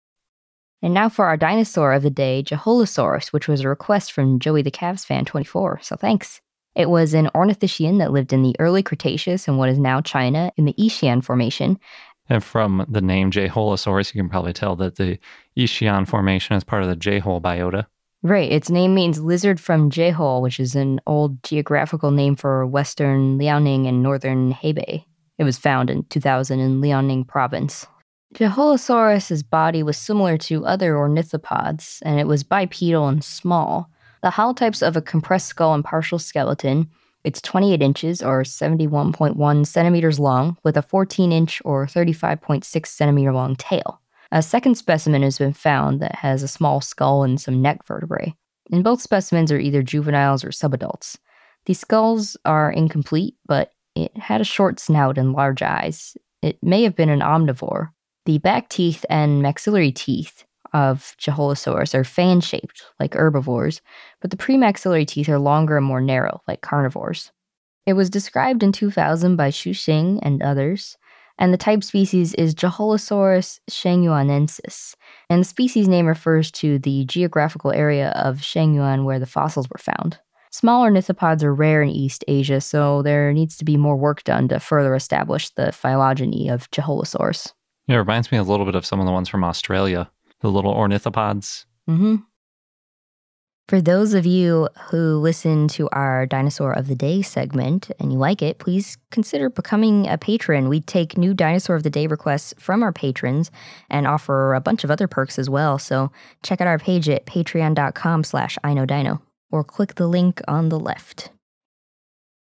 Jeholosaurus: Dinosaur of the Day
For the full episode where this dinosaur of the day was originally featured check out

Ornithischian that lived in the Early Cretaceous in what is now China (Yixian Formation in the Jehol Biota)
Body similar to other ornithopods
Bipedal and small
Holotype is of a compressed skull and partial skeleton, and is 28 in (71.1 cm) long, with a 14 in (35.6 cm) long tail
Second specimen found, has small skull and some neck vertebrae
Both specimens are either juveniles or subadults
Skulls are incomplete, but had a short snout and large eyes
May have been an omnivore
Back teeth and maxillary teeth are fan shaped, like herbivores
Premaxillary teeth are longer and more narrow, like carnivores
Found in 2000 in Liaoning Province and described in 2000 by Xu Xing, Wang Xioalin, and You Hailu
Type species is Jeholosaurus shangyuanensis
Name means "lizard from Jehol" (an old geographical name for western Liaoning and northern Hebei)
Species name refers to the geographical area of Shangyuan where the fossils were found
Small ornithopods are rare in East Asia. More work needs to be done to further establish the phylogeny of Jeholosaurus